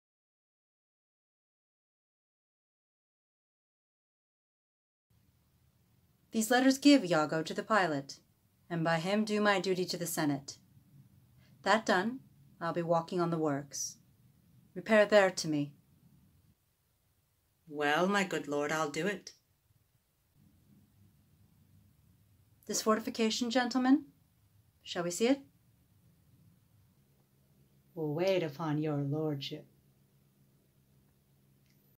Sofa Shakespeare: Othello - Act III Scene II
Cyprus. A room in the castle. This is a quick breather scene before the ridiculousness to come in Act III Scene III, that basically exists to set up that Othello wants Iago to meet him later after doing yet another servant-y job for him.

Characters in order of appearance: Othello; Iago; Gentleman

Credits:
1. Farnaz Ayrom-Walsh; London, UK